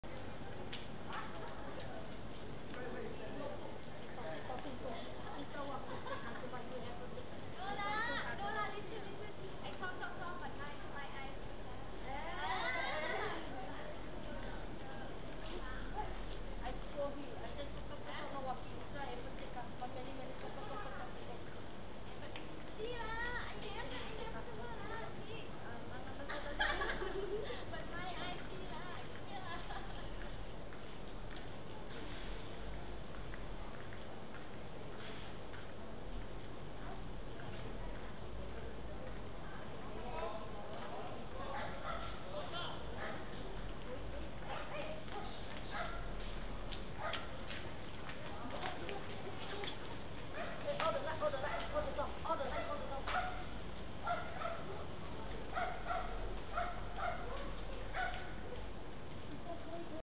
How the streetwalkers and pimps react when someone cries '"Police" - Part 1 (1 minute)
Two neighbouring units, i.e. No.8 (opposite my unit) and No. 9 (next to my unit) have prostitutes operating out of them. It's only one-plus in the morning and they are out in numbers, disturbing the peace. Towards the end of this clip, apparently they got news that something was amiss, a patrol car perhaps?